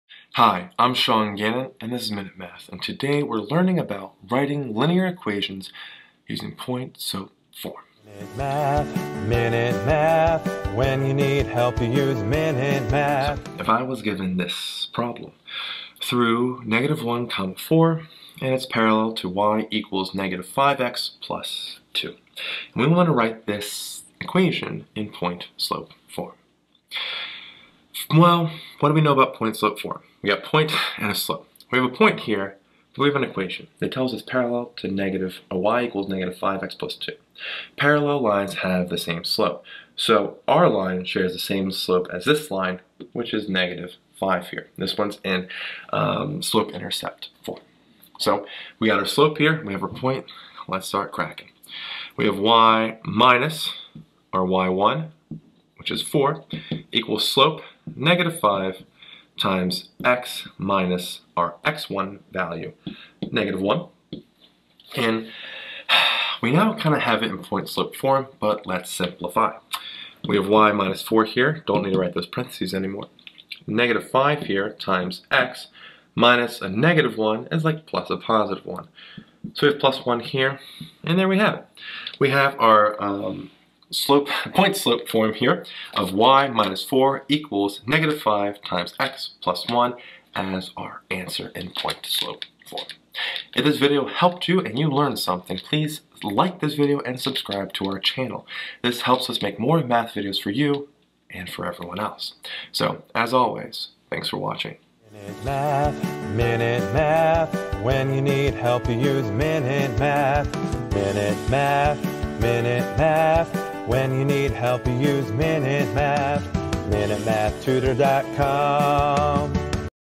Write the point-slope form of the equation: through (-1,4), parallel to y=-5x+2
In this math video lesson we Write the point-slope form of the equation: through (-1,4), parallel to y=-5x+2.